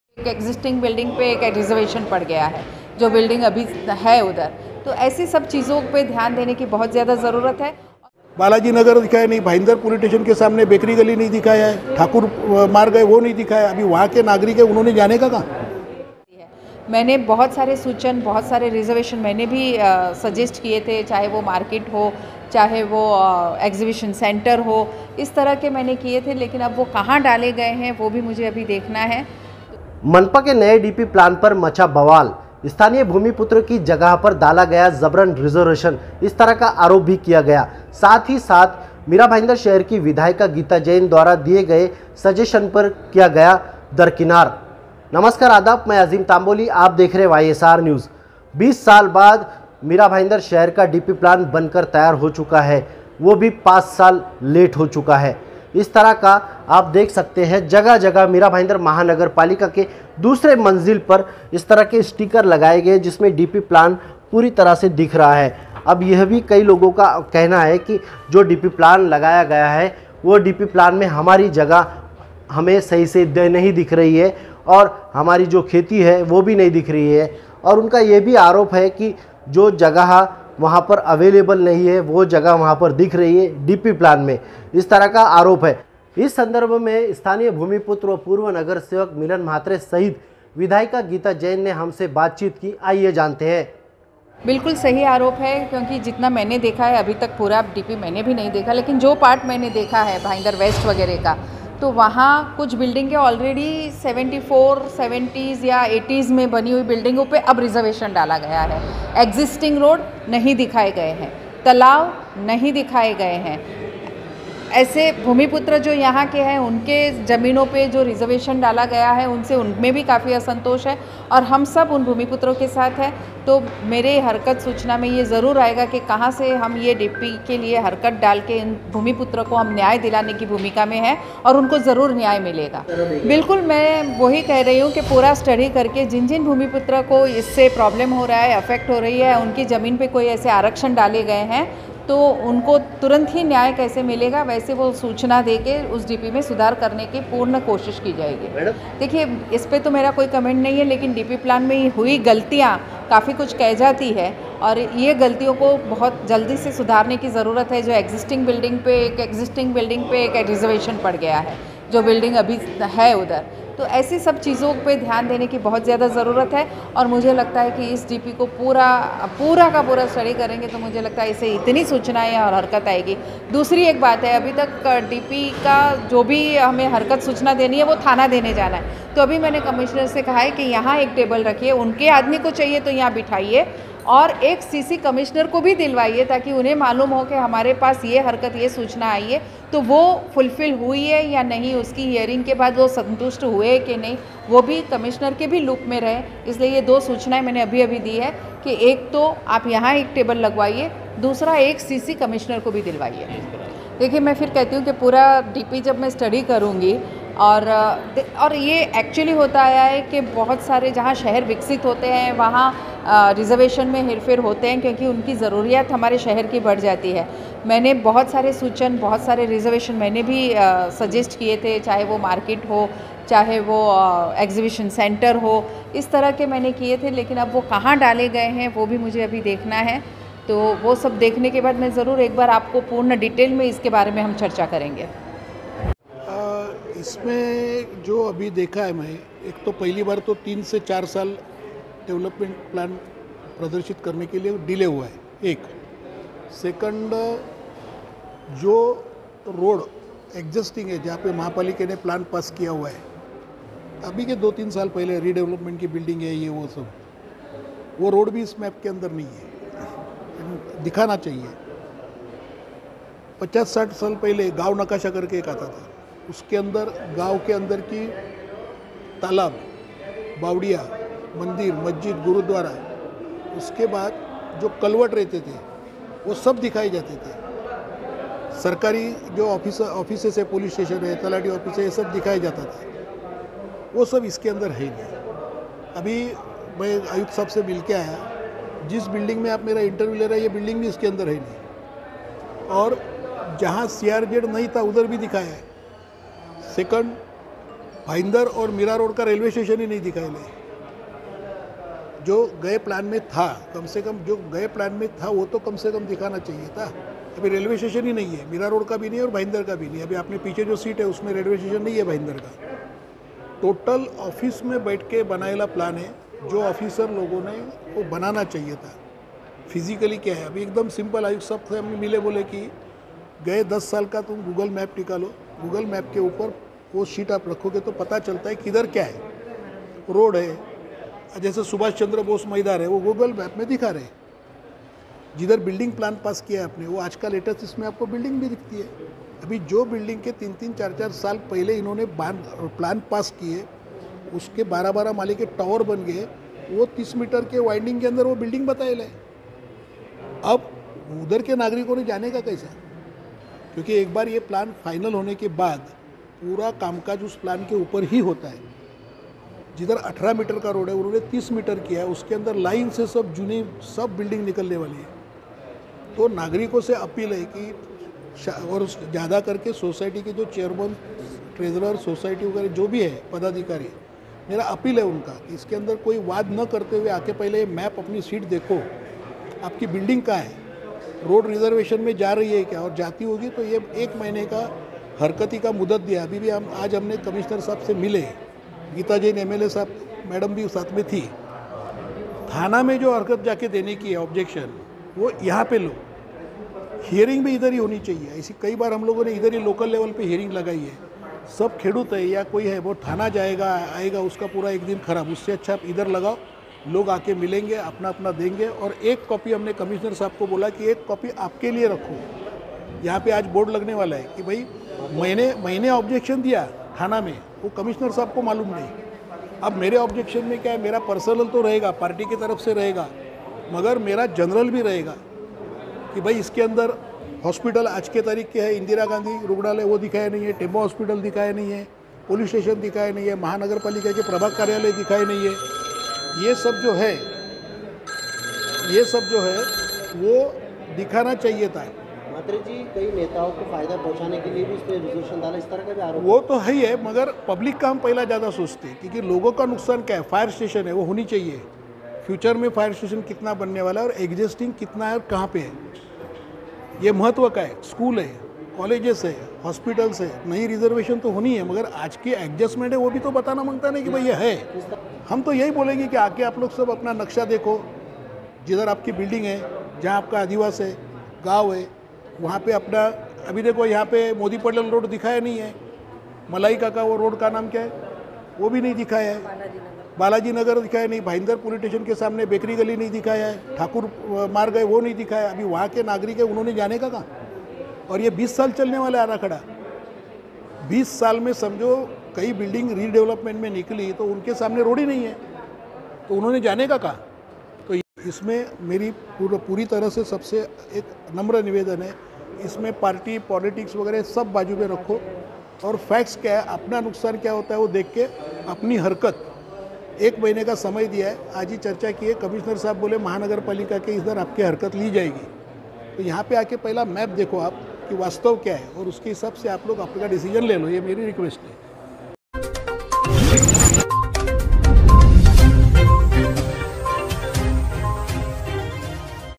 MBMC | मनपा के नए डीपी प्लान पर मचा बवाल-एसी कार्यालय में बैठ कर बनाया गया DP प्लान - नागरिक
मीरा भाईंदर शहर का dp प्लान 20 साल में आना था 5 साल हुई देरी...
स्थानीय भूमिपुत्रों में नाराजगी...
YSR NEWS
Mira Bhayandar DP plan